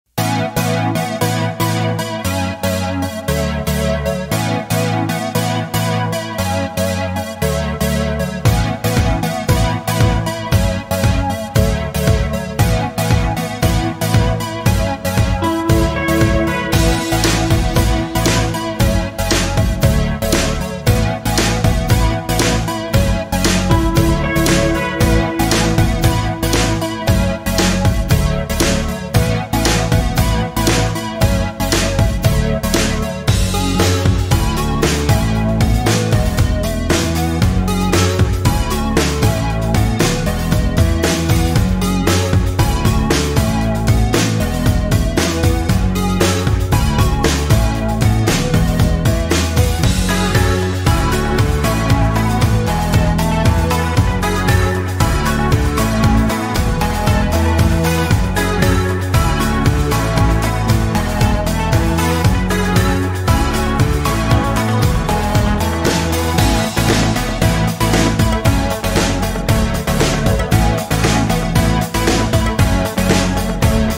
[4K] Indian AI Beauty Lookbook- Dense City with Pecan Trees
A maximalist, detailed CG landscape of a dense city with pecan trees, featuring stars in the sky and electric colors.